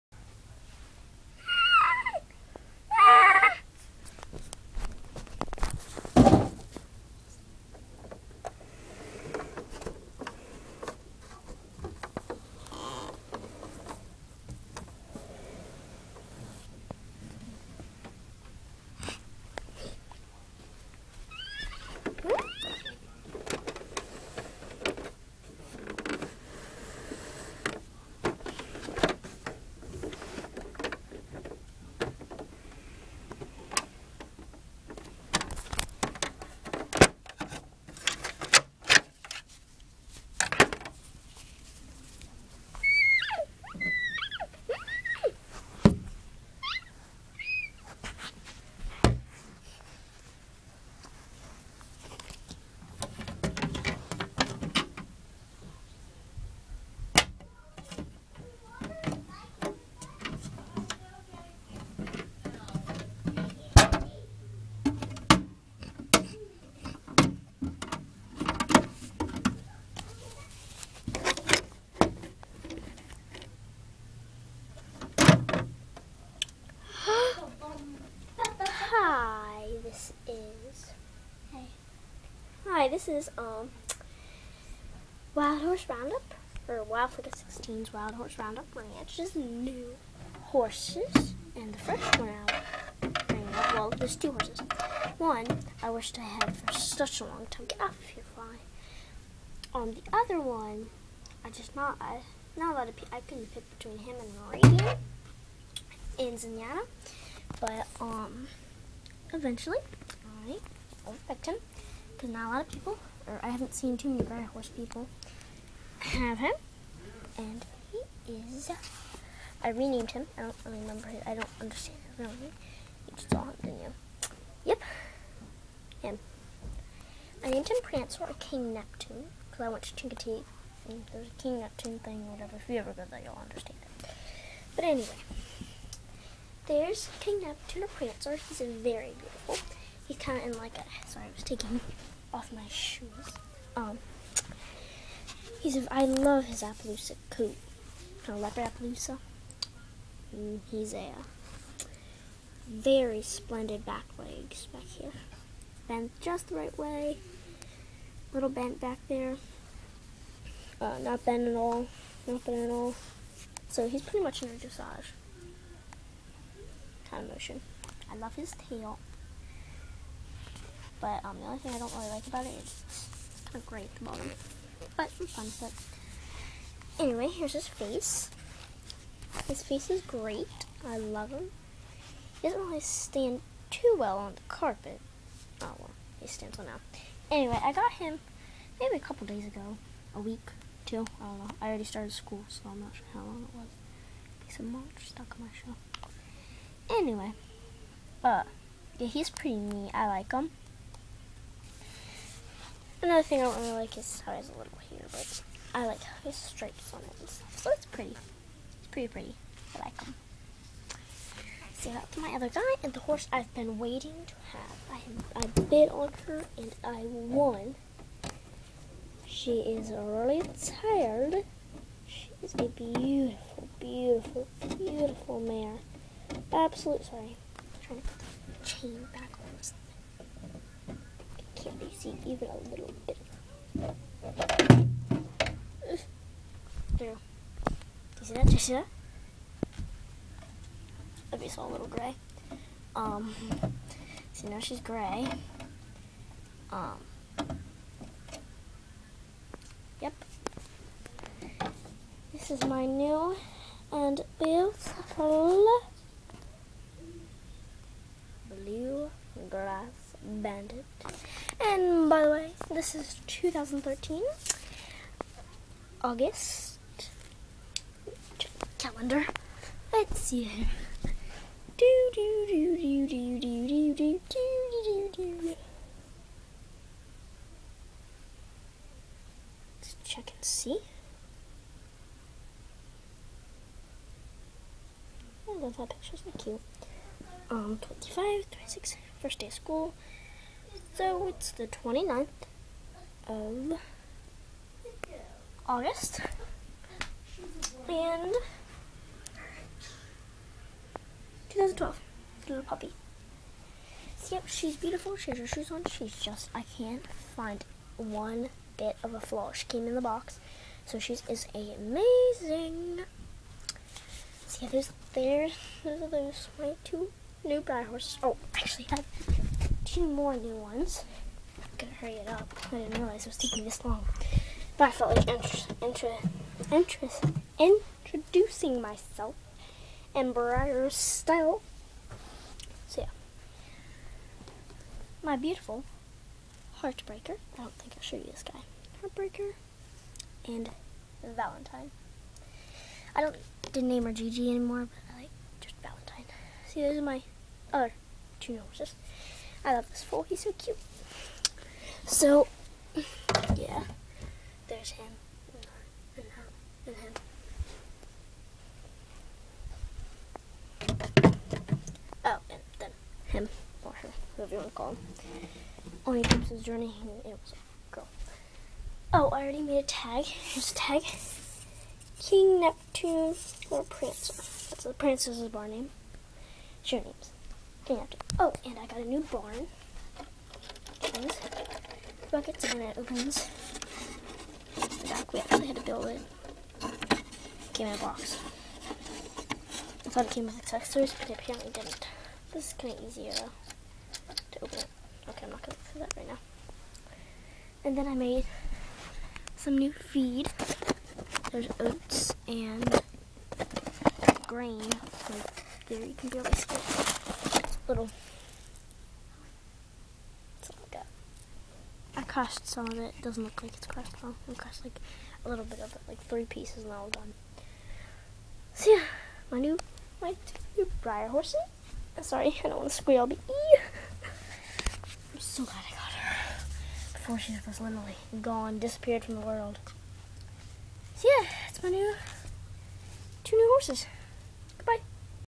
My new Breyer horses
Please comment,my favorite is blue grass bandit!I'm going to name her flicka for her barn name.The trailer is not that new.